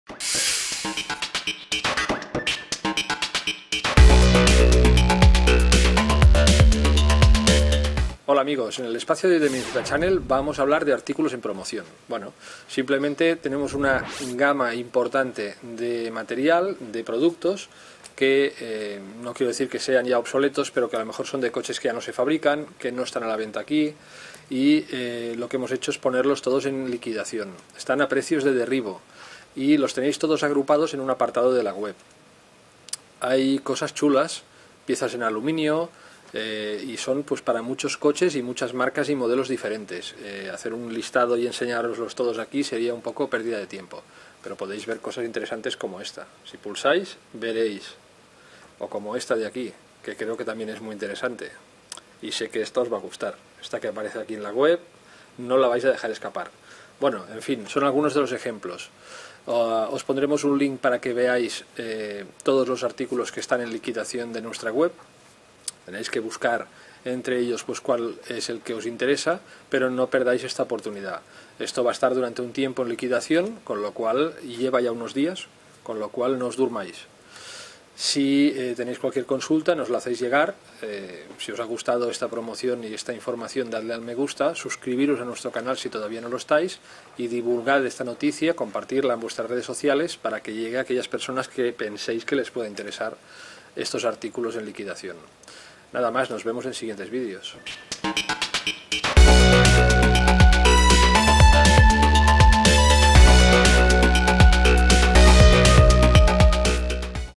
Liquidación de más de 500 artículos al 70% y 80% - Mini-Z Channel - 481 (w/Subtitles)
Buenos días minizeteros y amigos del radiocontrol!
Iniciamos la semana con este super ofertón por liquidación, con más de 500 artículos con descuentos del 70% y el 80%.

Aquí os dejamos el link para que podáis mirar la gran variedad de productos que tenéis:

¡No esperéis mucho en realizar vuestro pedido, porque son artículos que cuando se agoten no volveremos a tener! Y tu, ¿a qué estás esperando?